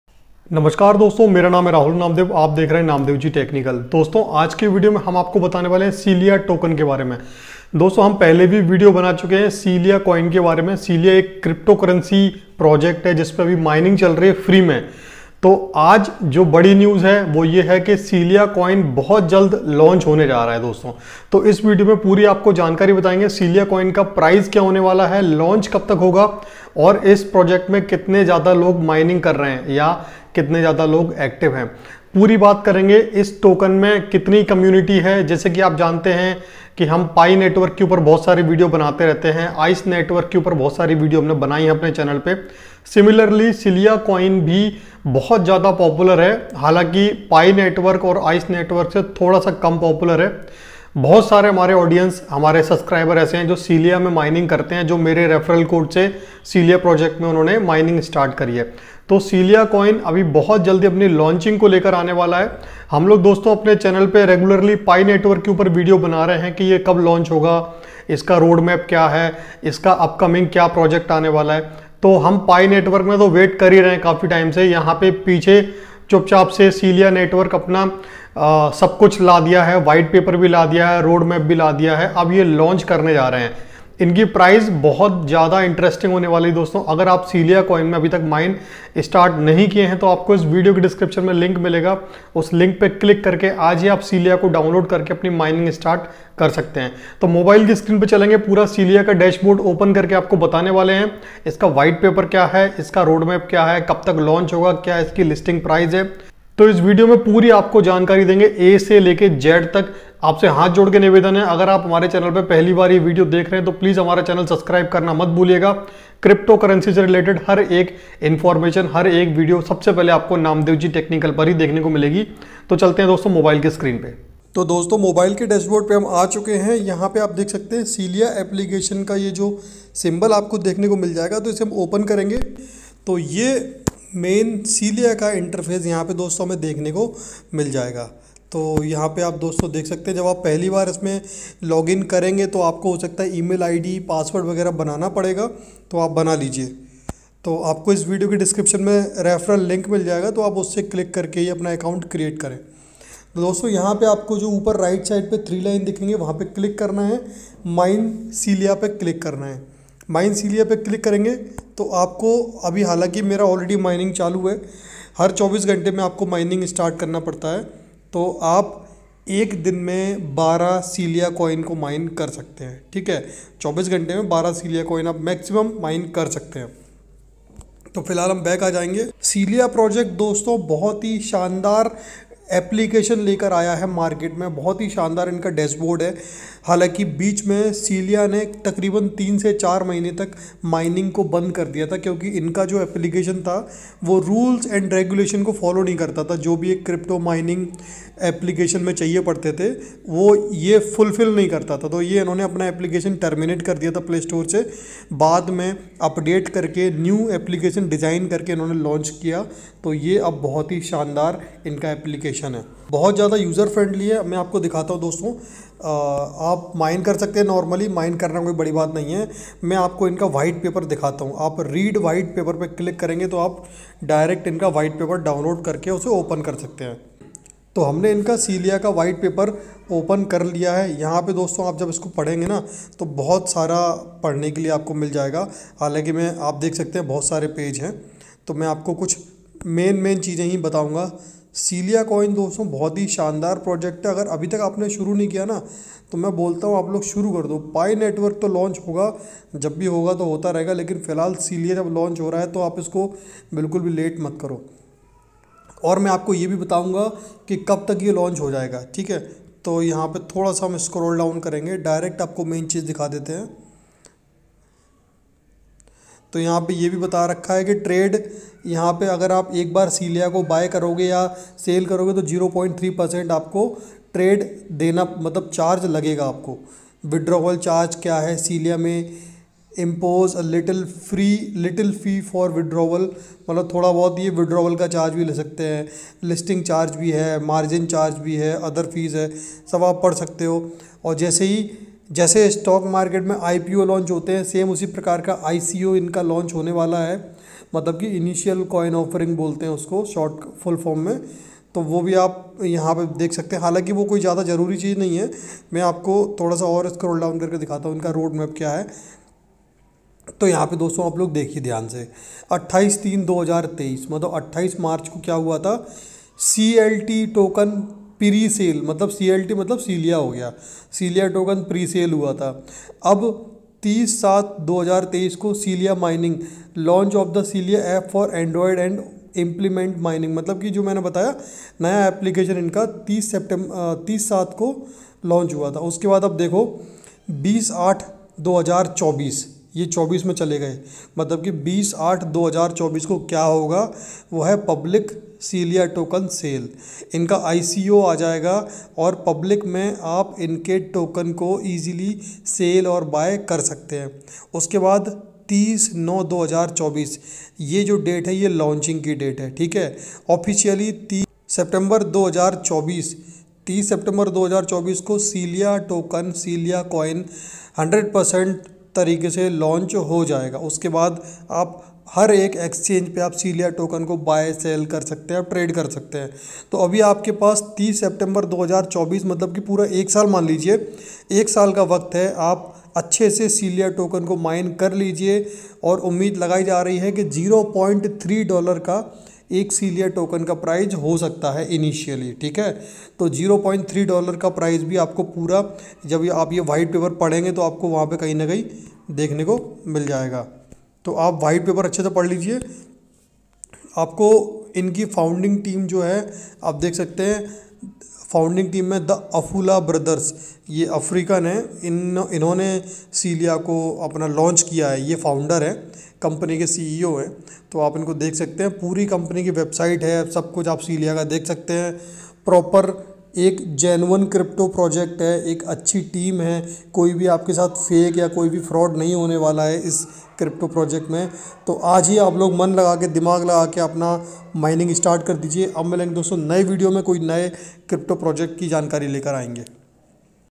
✅ Celia mining app launch date confirmed || Celia token open mainnet launch confirmed
Topics in video covered
hello friends in this video I will teach you about Celia crypto mining project, now Celia founder have confirmed the open mainnet launch date in Celia whitepaper. they have also launched the roadmap of launching the Celia token that known as CLT token. so just watch this video full to know about the Celia launching updates